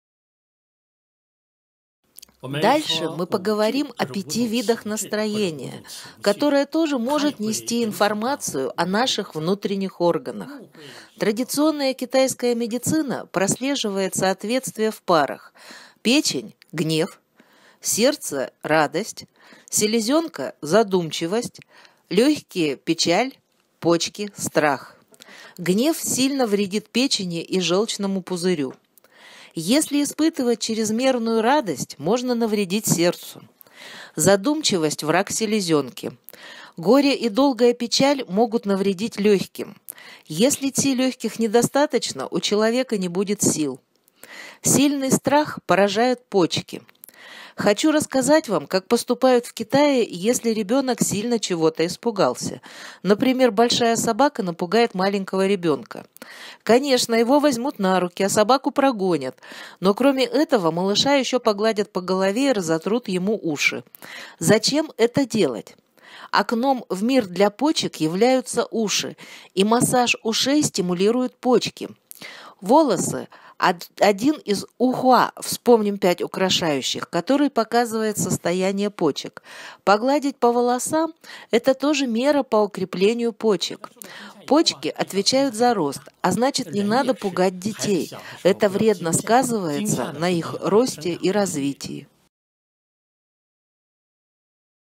КИТАЙСКАЯ ТИБЕТСКАЯ МЕДИЦИНА. ПЯТЬ ОРГАНОВ И ПЯТЬ НАСТРОЕНИЙ
ИЗМЕНЧИВОЕ НАСТРОЕНИЕ МОЖЕТ НЕСТИ ИНФОРМАЦИЮ О СОСТОЯНИИ ВНУТРЕННИХ ОРГАНОВ.
КИТАЙСКАЯ ТИБЕТСКАЯ МЕДИЦИНА ПРОСЛЕЖИВАЕТ СООТВЕТСТВИЕ В ПАРАХ.
ПЕЧЕНЬ - ГНЕВ
СЕРДЦЕ - РАДОСТЬ
СЕЛЕЗЁНКА - ЗАДУМЧИВОСТЬ
ЛЁГКИЕ - ПЕЧАЛЬ
ПОЧКИ - СТРАХ